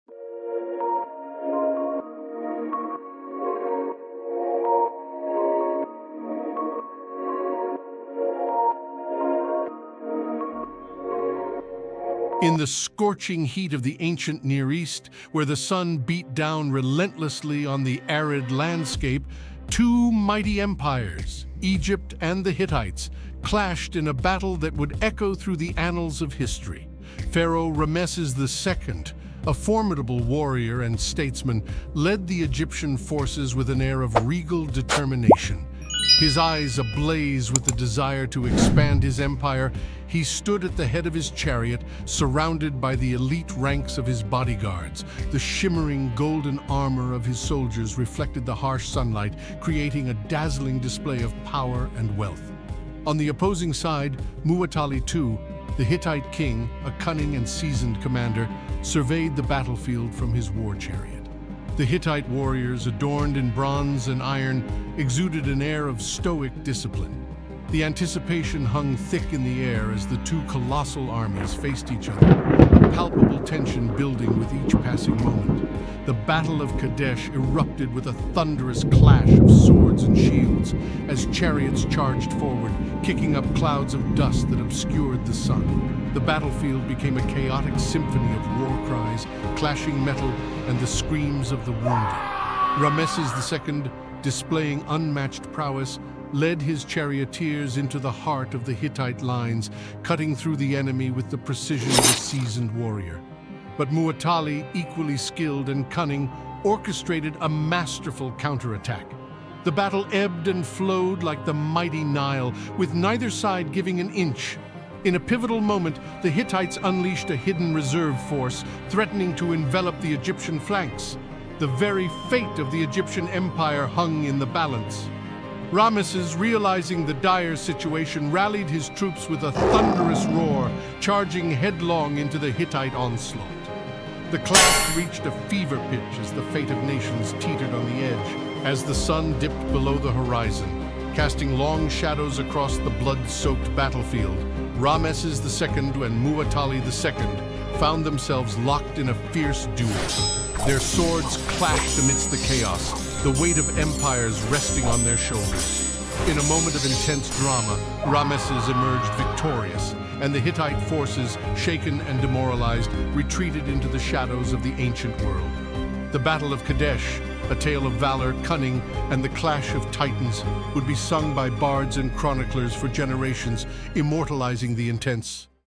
Epic Clash of Empires: Pharaoh Ramesses II vs King Muwatalli II | Battle of Kadass
Step back in time and witness the legendary Battle of Kadass, where Pharaoh Ramesses II and King Muwatalli II clashed in an epic struggle for supremacy. Join us as we delve into the historical significance, strategies, and outcomes of this monumental battle. Discover the secrets of ancient warfare and the impact it had on these mighty empires